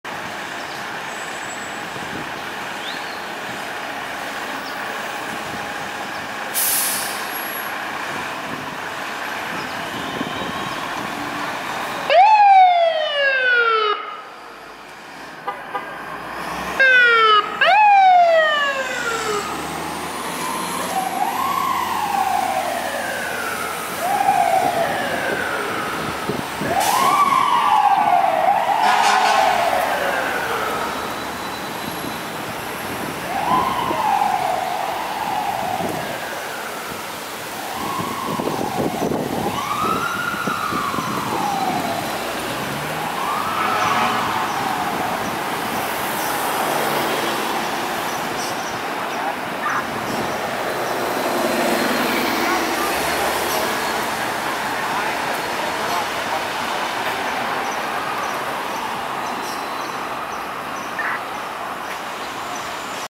FDNY Ladder 40 *2002 Spare PA -300*
FDNY Ladder 40 in a spare 2002 Tiller which is the oldest Tiller in the spare fleet Responding to an unknown Call

SL02004T - 2002 Seagrave Commander ii  100’ TDA ( Ex-CTS,  Ex- Ladder 20)